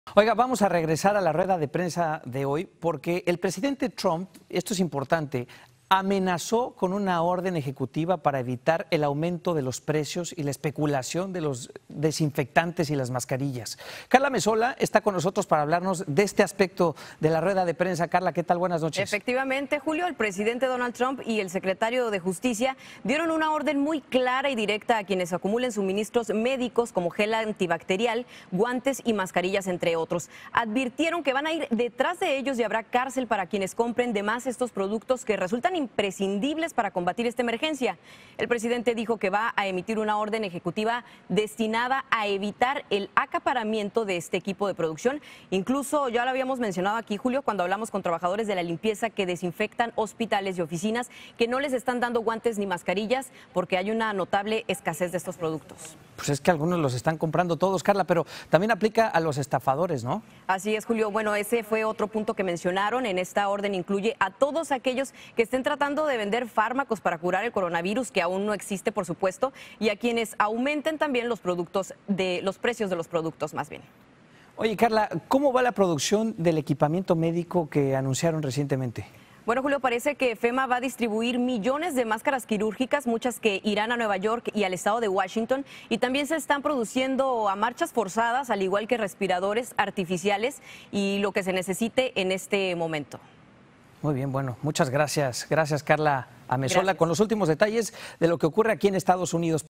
Anuncian duras medidas contra los acaparadores de productos de salud | Noticias Telemundo
Video oficial de Noticias Telemundo. El presidente Donald Trump y el Secretario de Justicia anunciaron duras medidas contra los acaparadores y especuladores de productos básicos de salud, necesarios durante la crisis del COVID-19.